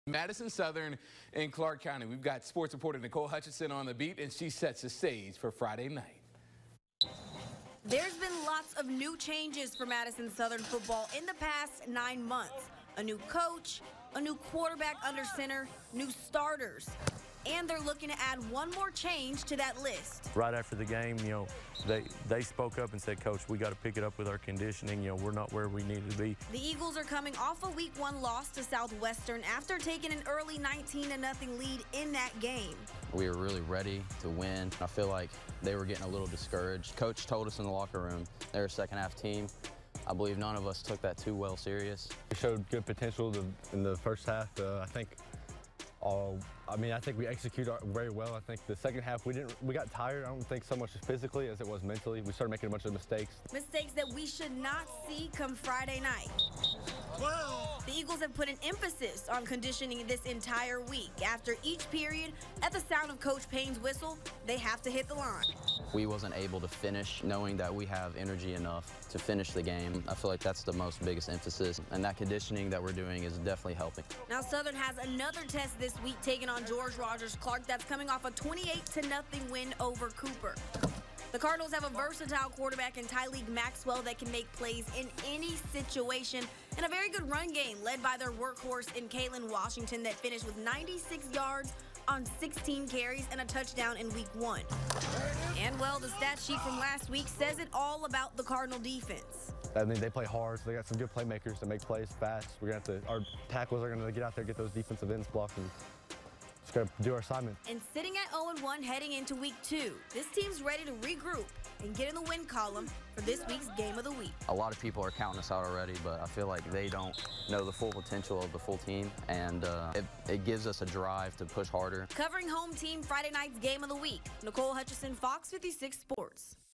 Madison Southern looking to get back in the win column
There's been a lot of new changes for Madison Southern football in the past nine months including hiring Mark Payne as the Eagles' new head football coach, giving Ethan Woods the starting job at quarterback, and putting new guys in starting positions.